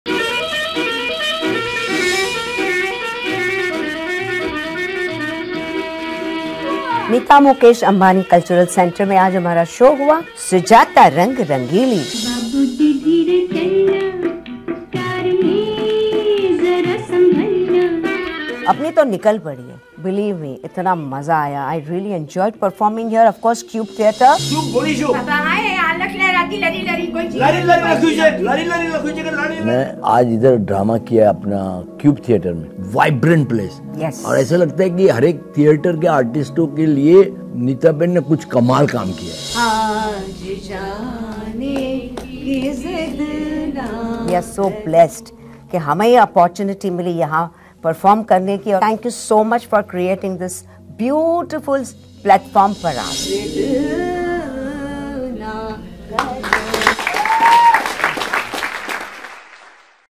Artists of 'Sujata Rang Rangili' Share Their Experiences At The Cube
Yesterday, we witnessed 'Sujata Rang Rangili' - a captivating one-woman play exploring the nine rasas of Indian classical art. Performed by Sujata Shah and written and directed by Latesh Shah, this powerful Gujarati production enthralled us with its immersive theatricality and Shah's compelling performance. Celebrated for its rich portrayal of emotions, the play left a lasting impression, blending cultural authenticity with contemporary storytelling. Watch the artists share their experiences at TheCube.